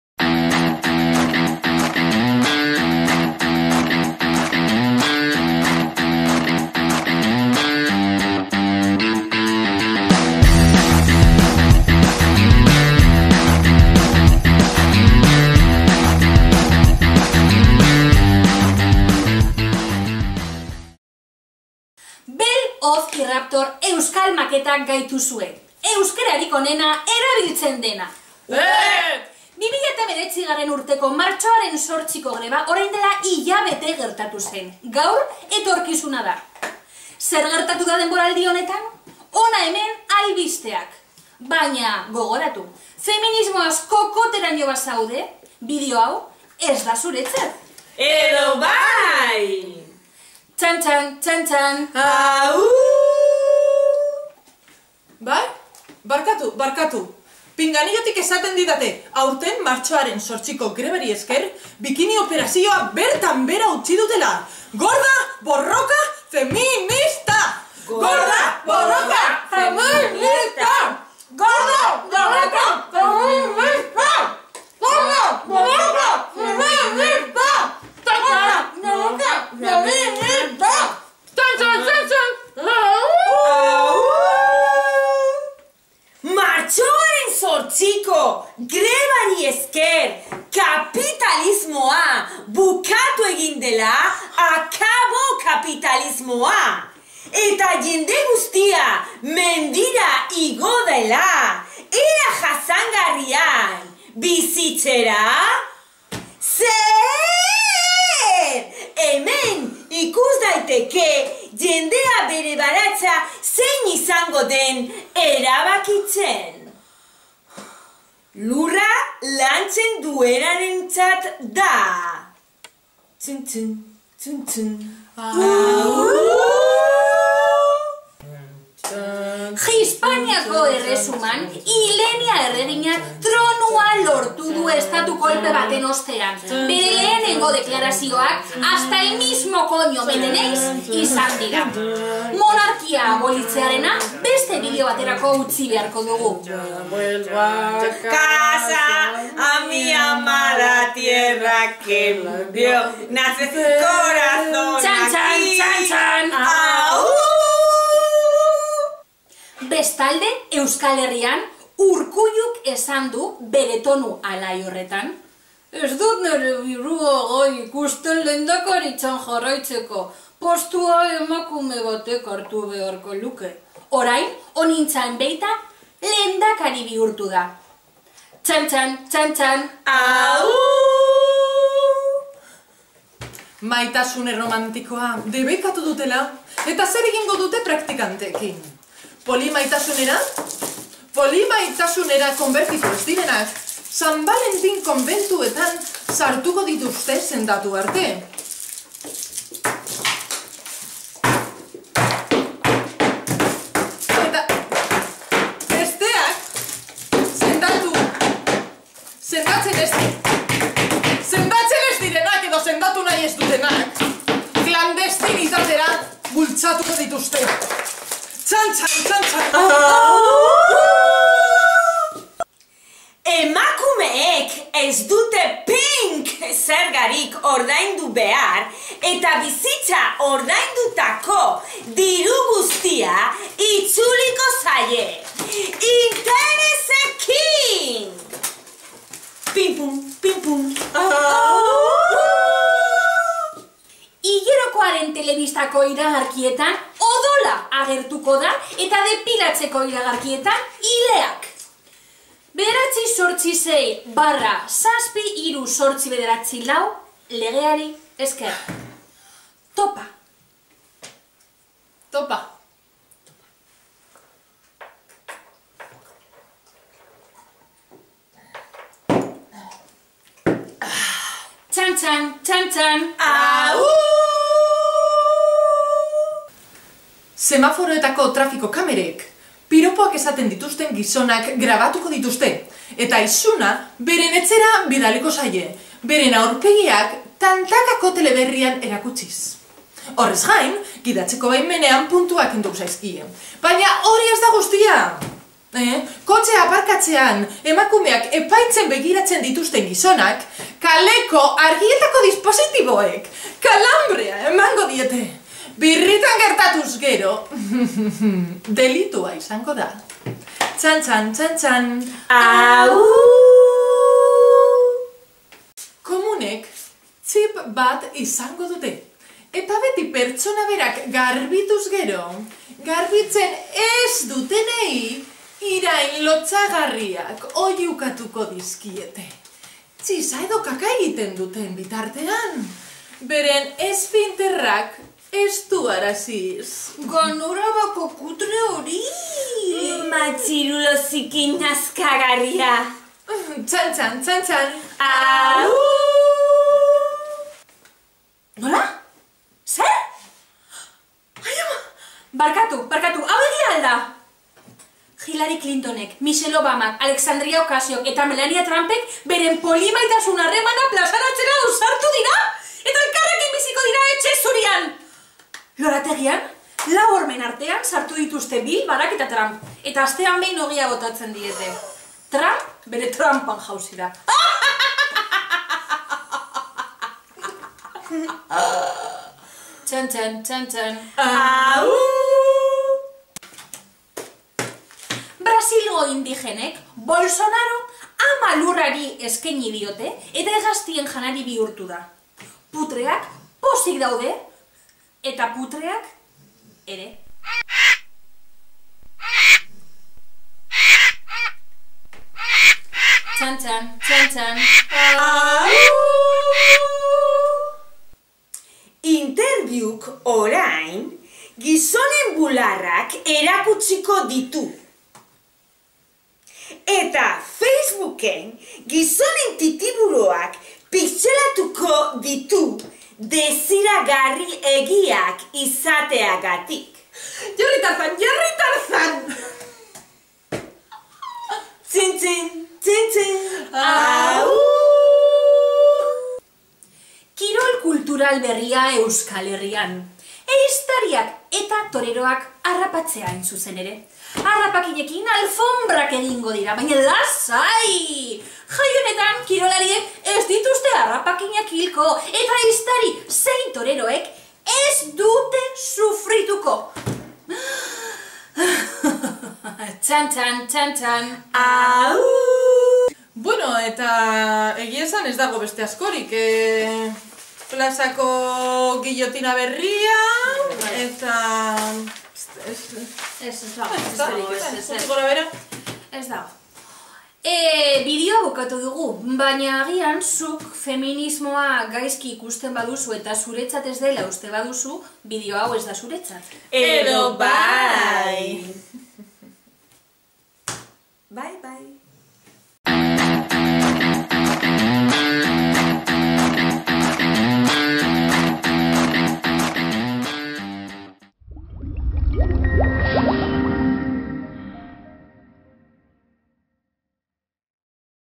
Emakume* guztiok itxaroten dugun teleberria
Jendeartean aldaketa batzuk gertatzen ari dira, Bell Hoz y Raptorrek teleberri aurkezle bihurtu dira eta kontatuko dizkigute aldaketa hauen berriak.